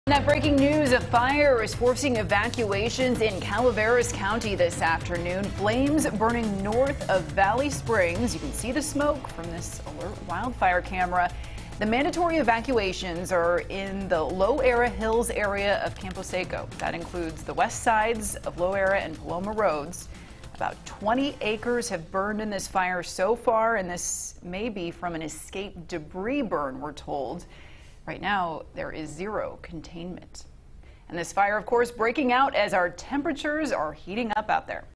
Growing Calaveras County Fire Forces Evacuations
The fore was burning north of Valley Springs.